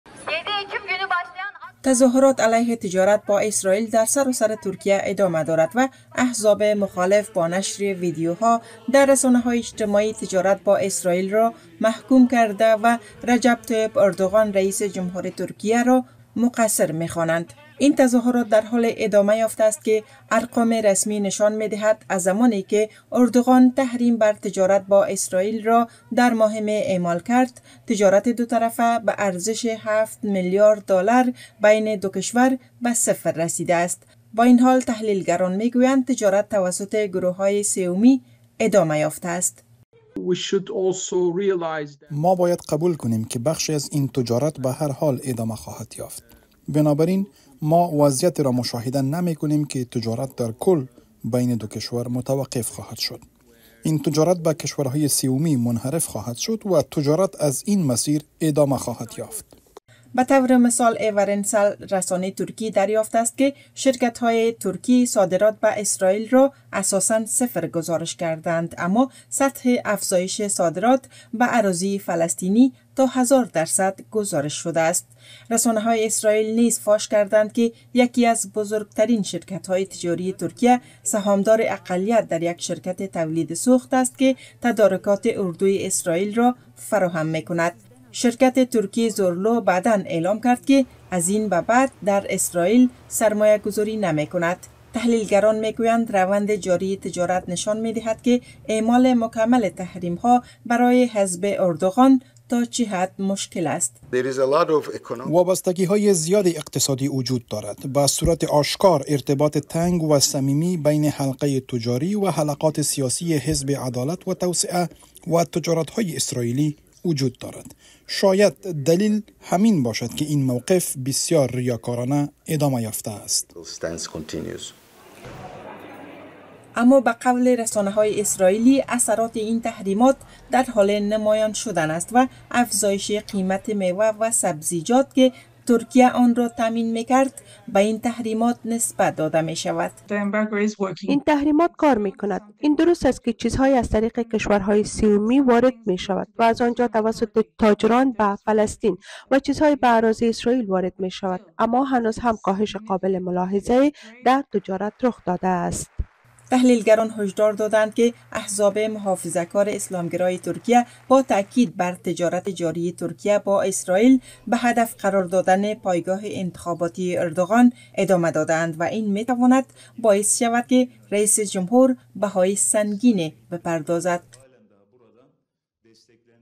اعتراض بر تطبیق نشدن تحریم‌های ترکیه بر تجارت با اسراییل
در حالی که بررسی دقیق تحریم‌های ترکیه بر تجارت با اسراییل افزایش یافته است، مخالفان ادعا می‌کنند که این تحریم‌ها به گونهٔ  قابل ملاحظه دور زده می شود. این اتهامات در حالی مطرح می‌شود که رجب طیب اردوغان، رییس جمهور ترکیه در تلاش ایفای نقش رهبری کننده در مخالفت با جنگ اسراییل علیه حماس است.